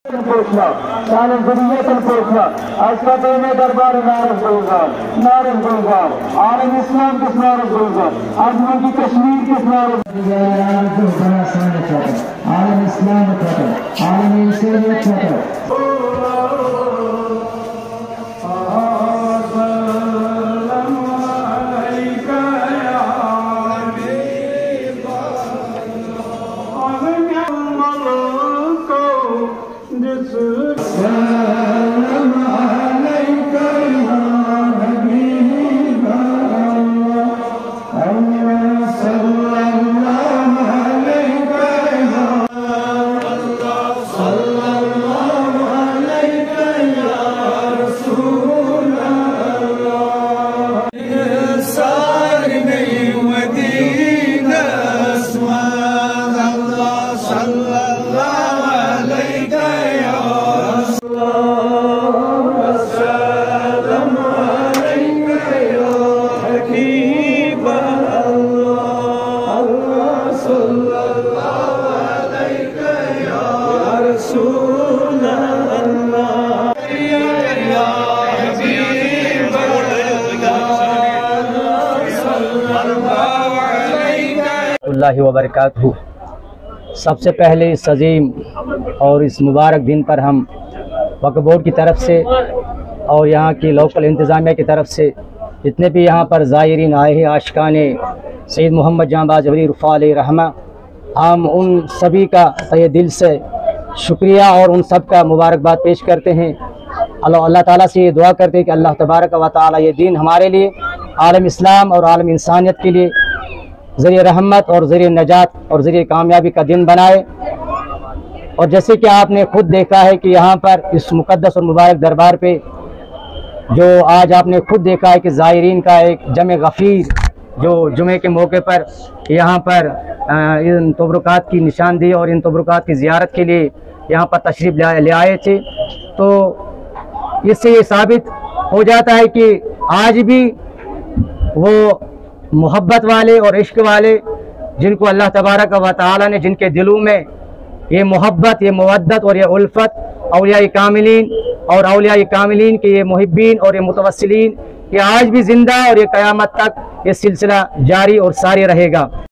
605th Urs Mubarak of Hazrat Syed Muhammad Janbaz Wali Alihi Rehma at khanpora Baramulla.
605th Urs Mubarak of Hazrat Syed Muhammad Janbaz Wali Alihi Rehma at khanpora baramulla Holi relics were displayed.
The annual Urs of Hazrat Syed Janzab Wali (RA) was celebrated at Syed Janbaz Wali shrine with great devotion and religious enthusiasm. Devotees including elders, men, women, and children gathered here to have a glimpse of “Tubrookat” holy relics after every Nizmaz (prayer).

While paying their obeisance at the Shrine devotees prayed for peace and prosperity in the Union Territory and the Muslim world. SSP baramulla along with other seniors officers visited the dargah meanwhile Intizamiya committee thanked administration and waqf board for the arrangements